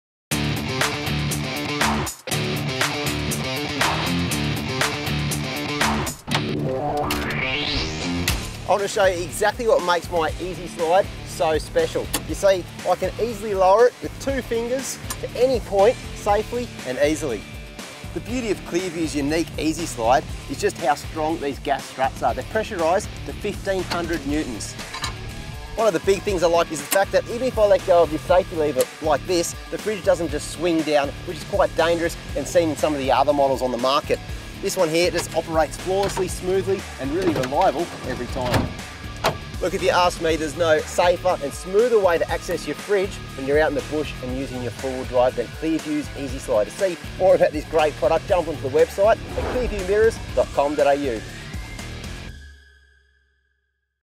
4WD - Clearview Accessories - Easy Slide holds 180kg NO TROUBLE !
WHAT DOES YOUR FRIDGE WEIGH WHEN FULLY LOADED? The Easy Slide can hold 180kg no trouble! Clearview Accessories over engineer their products to ensure a safe, long working life.